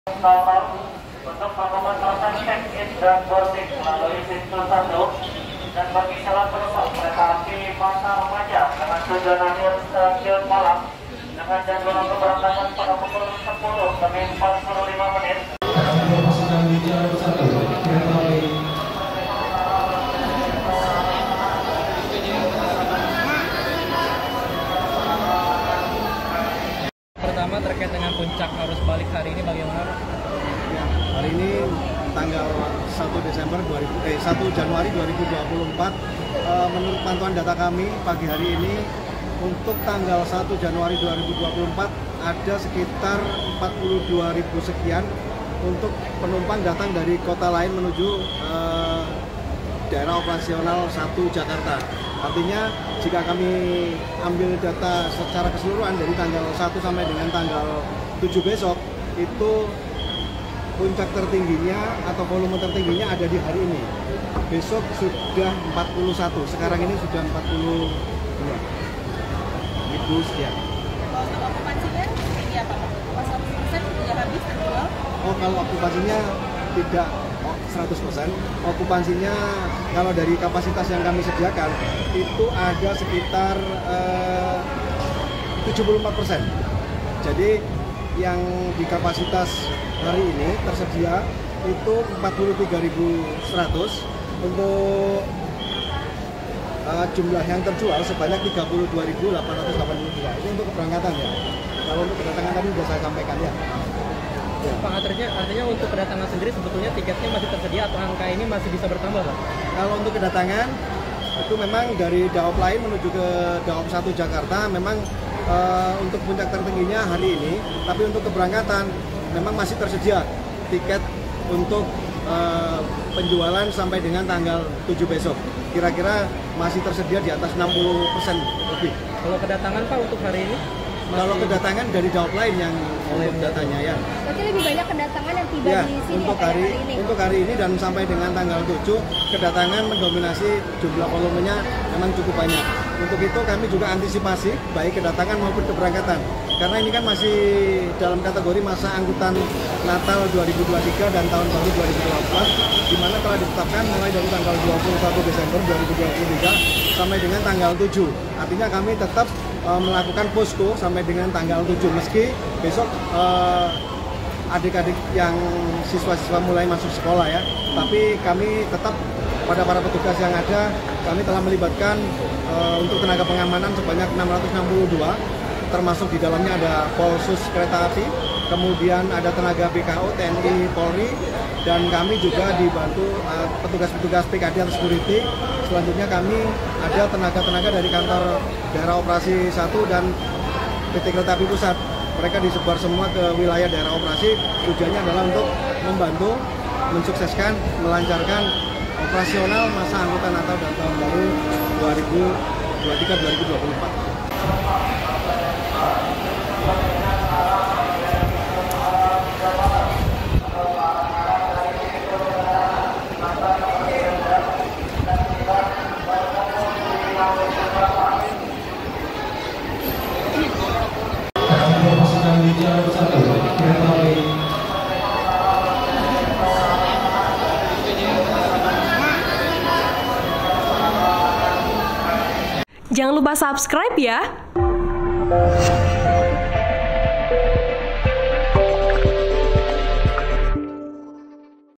Lonjakan Arus Balik Nataru Tertinggi di Stasiun Pasarsenen, PT KAI Sebut Seperti 2019
WARTAKOTALIVE.COM, Jakarta -

Hari terakhir musim libur Natal dan tahun baru 2024, Stasiun Pasarsenen, Jakarta Pusat, ramai didatangi penumpang yang kembali pulang dari kampung halamannya, Senin (1/1/2024).
Pantauan Wartakotalive.com di lokasi, nampak ribuan penumpang silih berganti keluar dari pintu kedatangan Stasiun Pasarsenen sembari menenteng sejumlah koper dan tas-tas berukuran besar.
Sejumlah pelukan hangat dari keluarga yang menjemput pun terlihat di lobi pintu masuk stasiun.
Beberapa porter juga nampak sibuk mengangkut barang-barang bawaan penumpang ke mobil-mobil jemputan yang masuk ke area Stasiun Pasarsenen.
Menurut Manager Humas PT Kereta Api Indonesia (KAI) Daop 1 Jakarta, Ixfan Hendriwintoko, hari ini merupakan volume kedatangan penumpang terbanyak pada musim libur Nataru.
"Untuk tanggal 1 Januari 2024, ada sekitar 42.708 penumpang datang dari kota lain menuju Daop 1 Jakarta," kata Ixfan saat ditemui di Stasiun Pasarsenen, Jakarta Pusat, Senin.
Jurnalis Video    : Nuri Yatul Hikmah